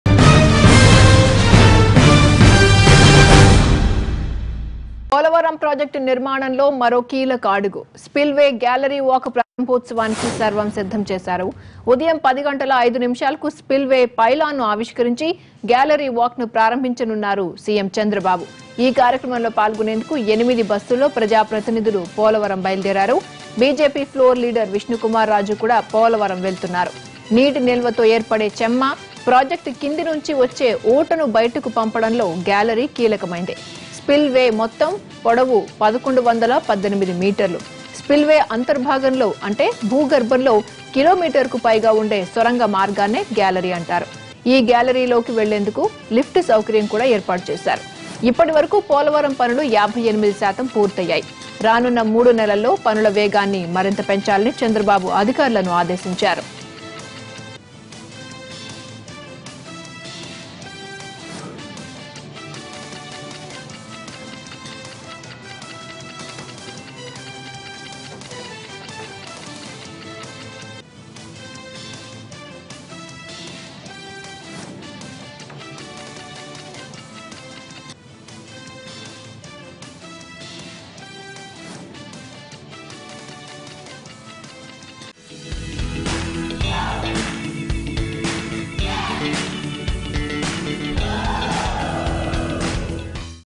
CM Chandrababu Naidu to Launch Polavaram Spillway Gallery Walk Today | NTV
CM Chandrababu Naidu will be visiting the Polavaram Project site to To Inspect spillway gallery work and to Launch Polavaram Spillway Gallery Walk Today, Chandrababu Naidu to see the progress of the gallery walk of the project more than 58% of the Polavaram project works have been completed.